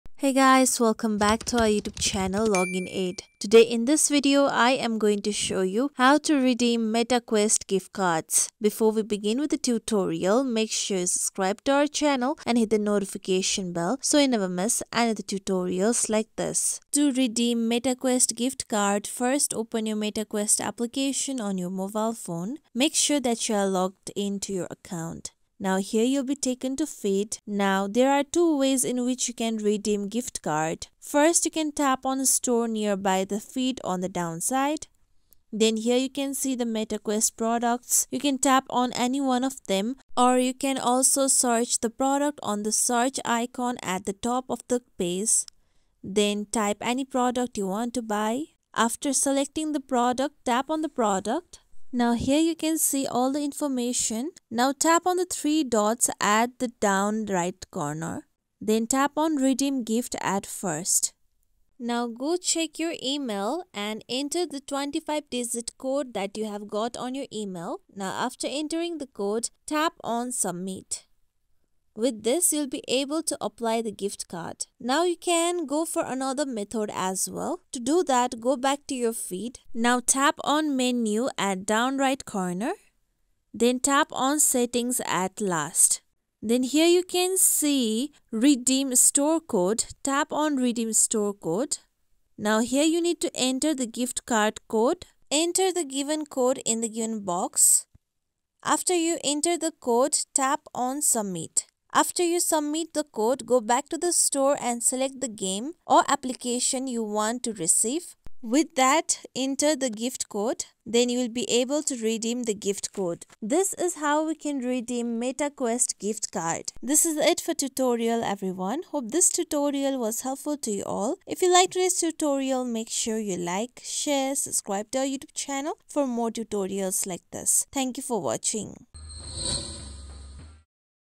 How To Redeem Meta Quest Gift Card 2024 | Use Meta Quest Gift Cards
A video guide for the process to redeem and use Meta Quest Gift Card.